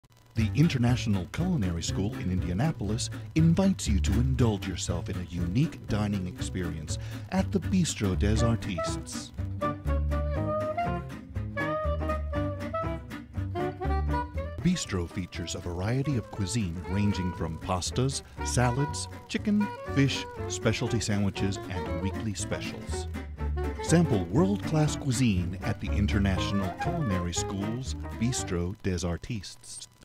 Bistro des Artistes
Introduction to Video class - We had to create 30 second commercial advertising a real product or service.  We decided to promote the restaurant at the Culinary School.